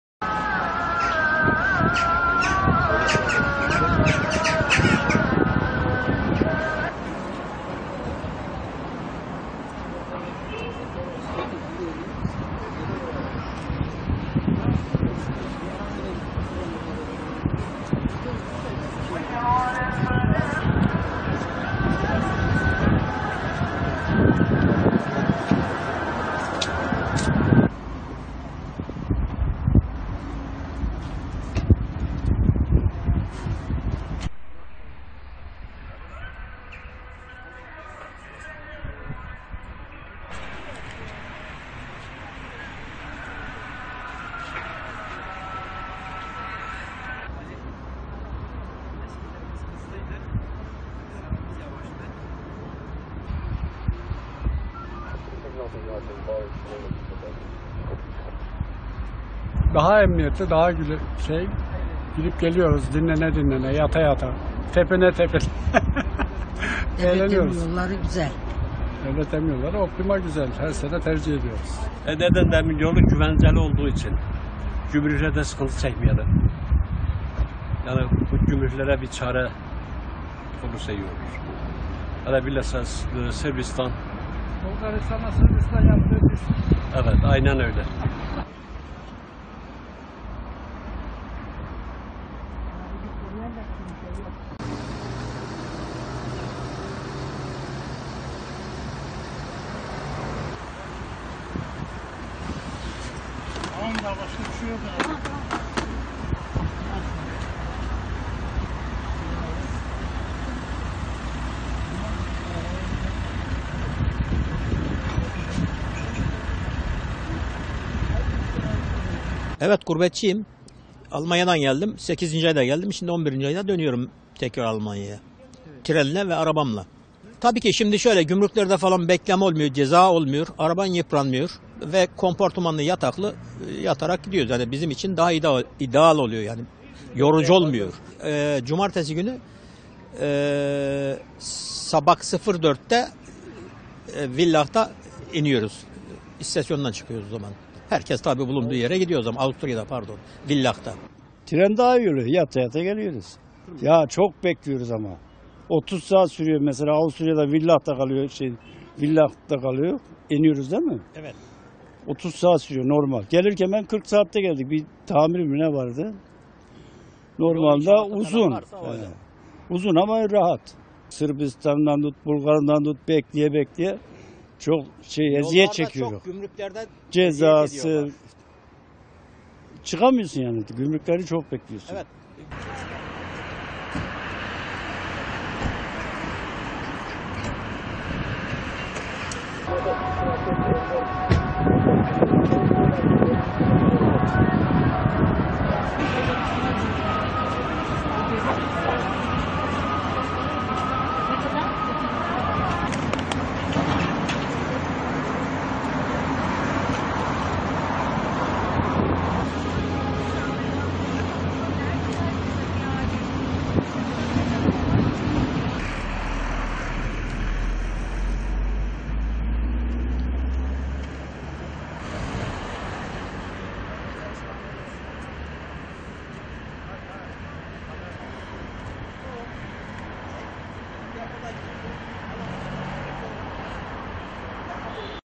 Yaz tatili sona eren gurbetçiler geri dönüş için arabalı treni tercih ediyor
Emekli gurbetçilerin geri dönüşte gözdesi tren yolculuğu: Yata yata gidiyoruz!